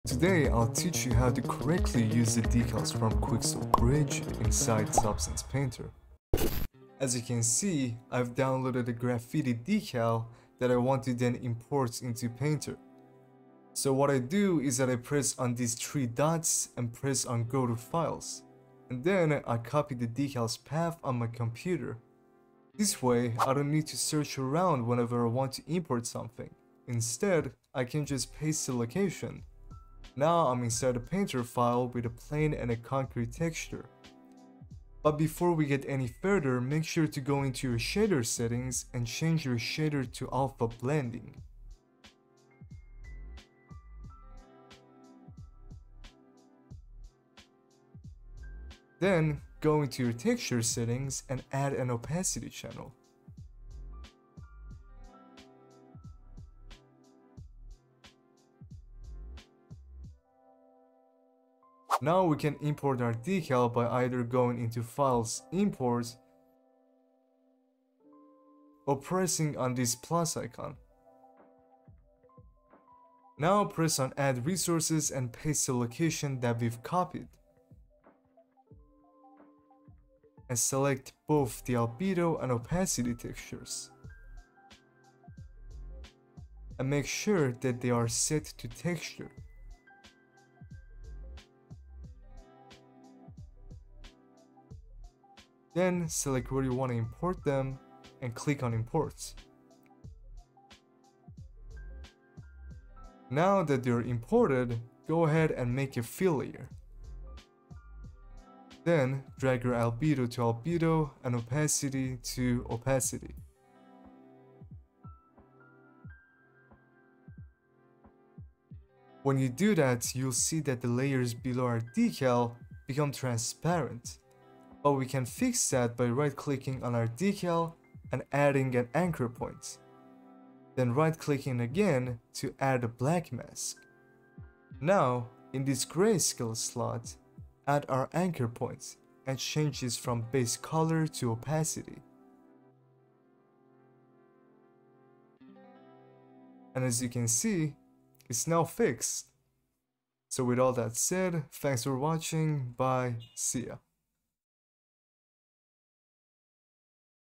How to use Quixel Decals inside Substance Painter!
in today's video, I'll teach you how to use the Decals and Graffitis that you've downloaded from Quixel, inside Substance Painter

🖥PC SPECS🖥
GPU►Nvidia GeForce RTX 2060 SUPER 8G
CPU►Intel® Core™ i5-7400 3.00 GHz
RAM►16GB